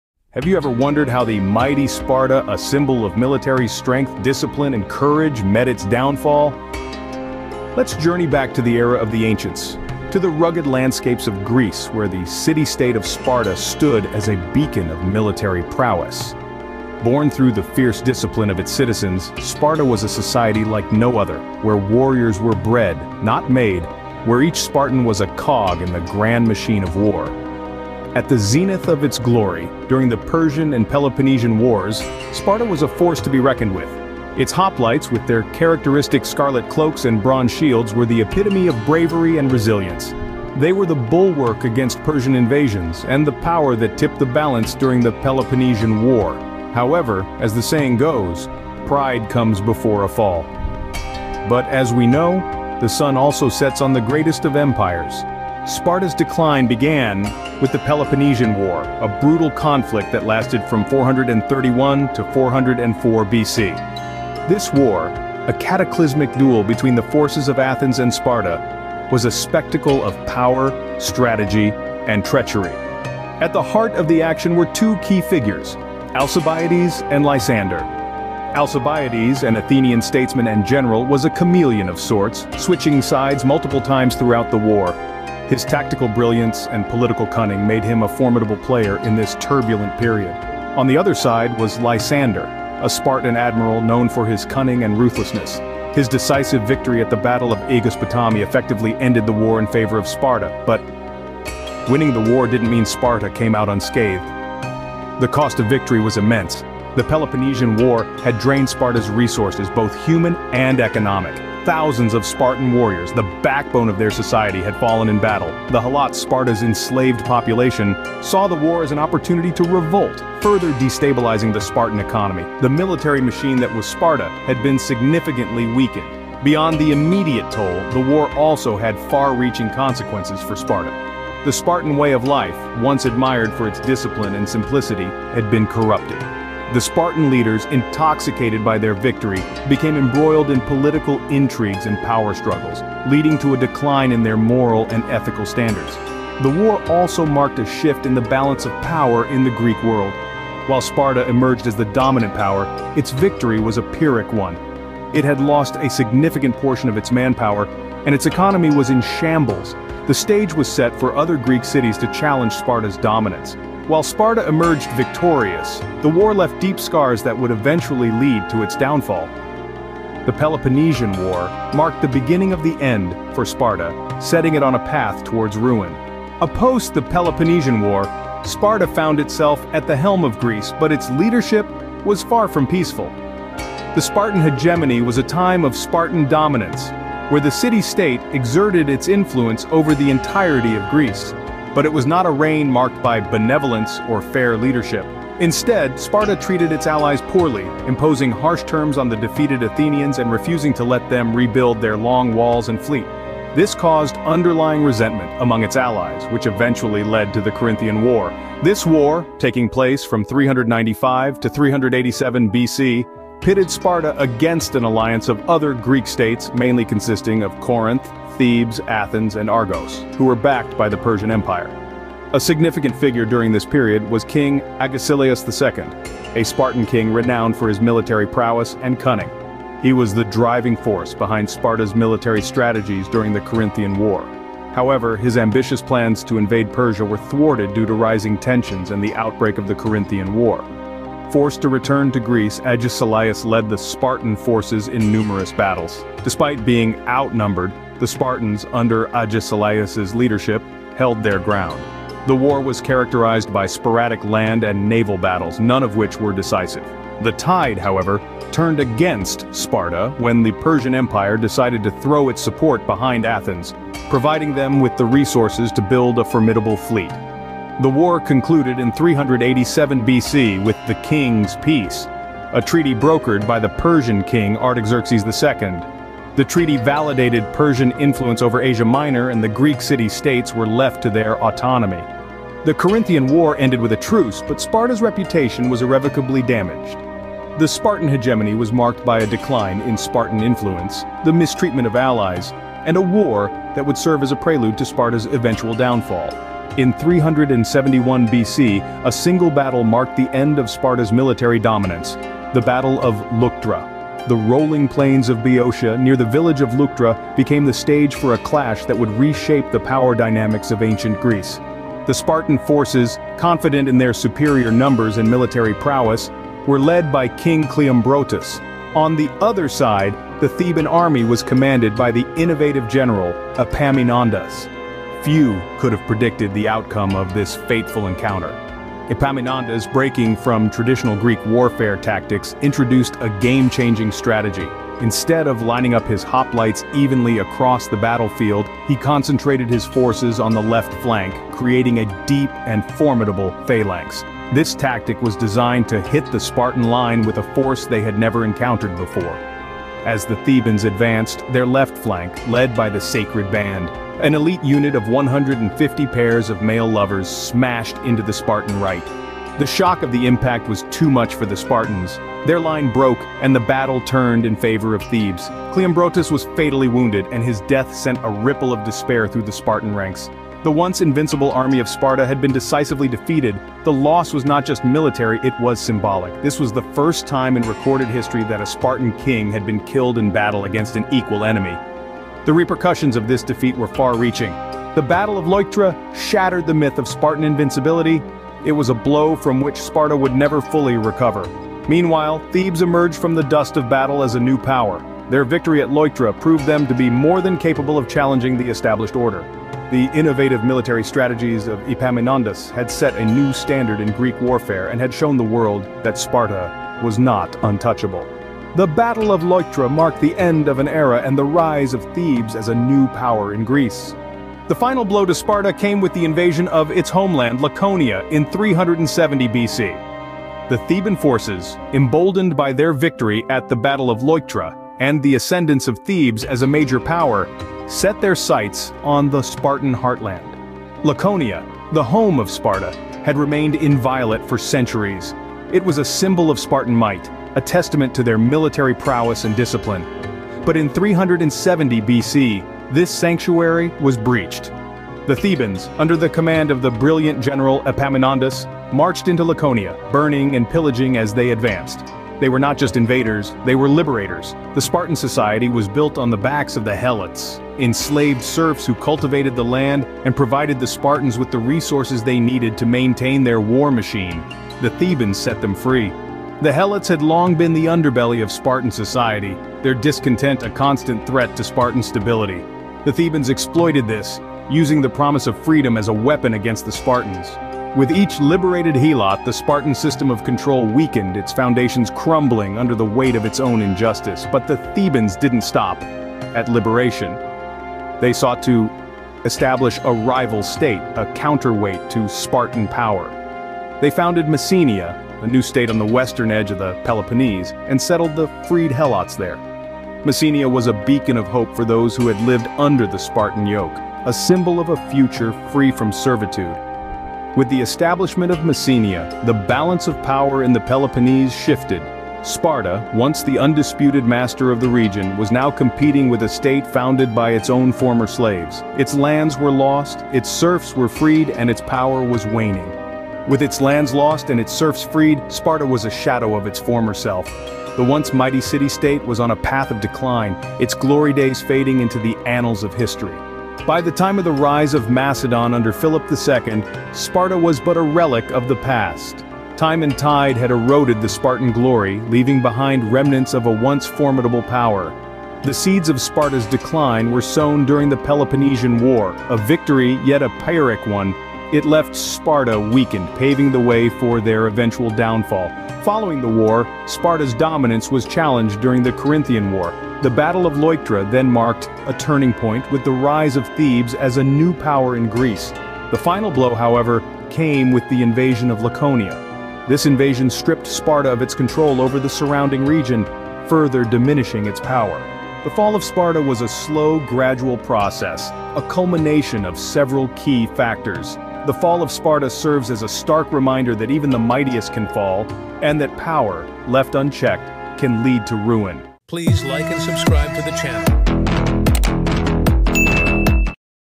The Epic Saga of Sparta | Rise and Fall Unveiled!
In this video, we delve into the rise and fall of Sparta, one of the most powerful city-states in ancient Greece. From its glory days as a military powerhouse to its eventual decline and ultimate downfall, we examine the key events and decisions that led to the demise of this once-mighty civilization. Join us as we unravel the fascinating story of Sparta's journey from greatness to ruin.